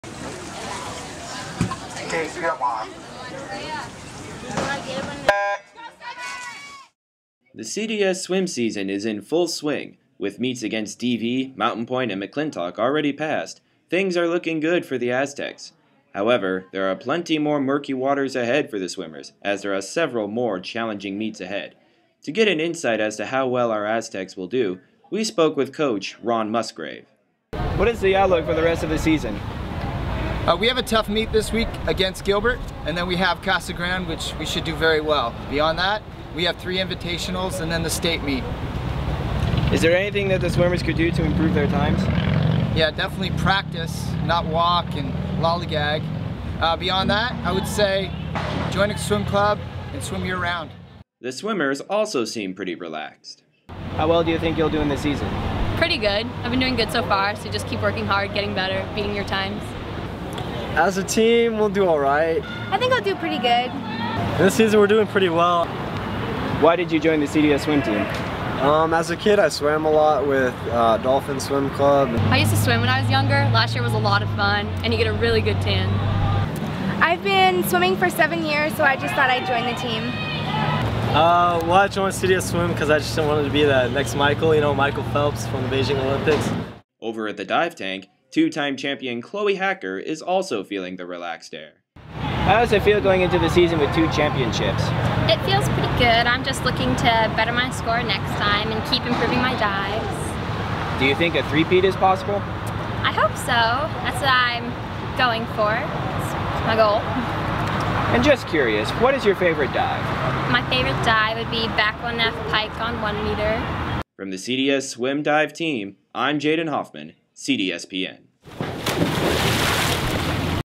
Swim & Dive Report
Jaden checks in on Corona's Swim and Dive team